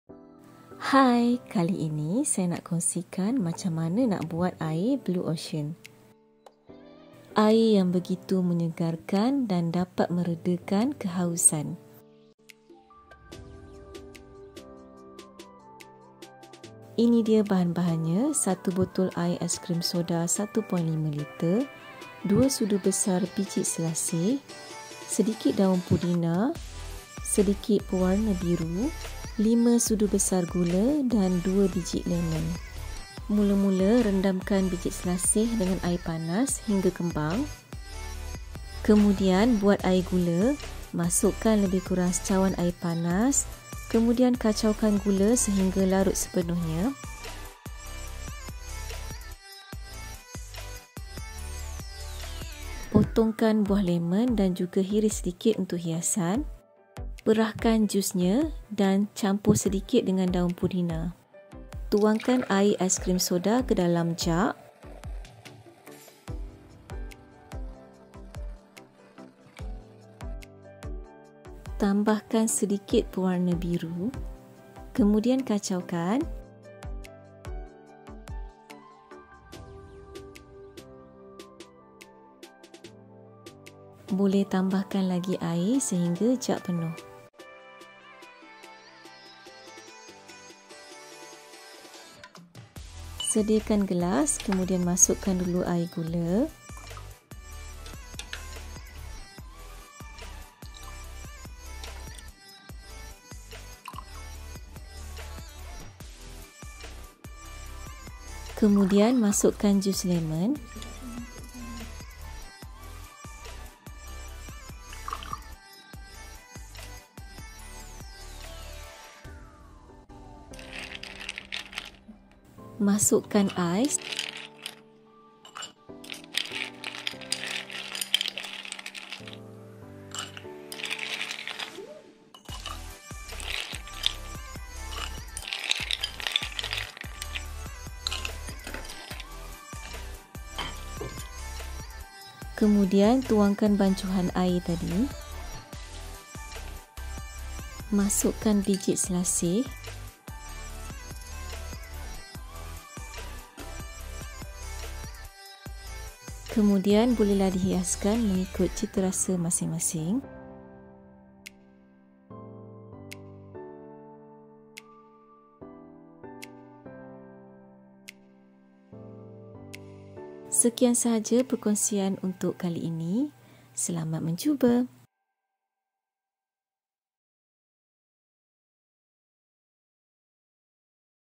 Air Blue Ocean | MENYEGARKAN | Mudah disediakan
Bahan-bahan

2 sudu besar biji selasih
2 biji lemon
Sedikit daun pudina
1 botol air ais krim soda(1.5 liter)
Sedikit pewarna biru
5 sudu besar gula

Musician: @iksonmusic